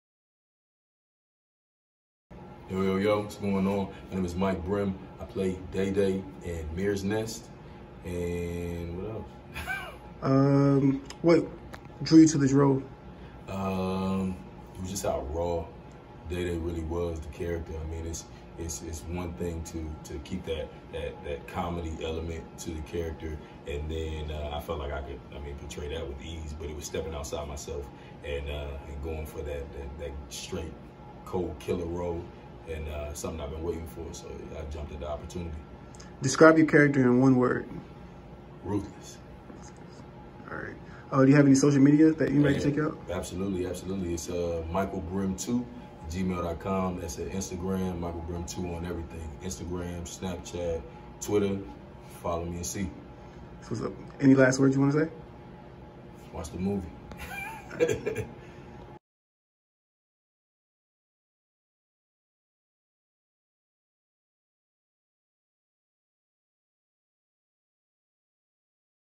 Michael Brim
Actor Michael Brim on his character grim in the film mares nest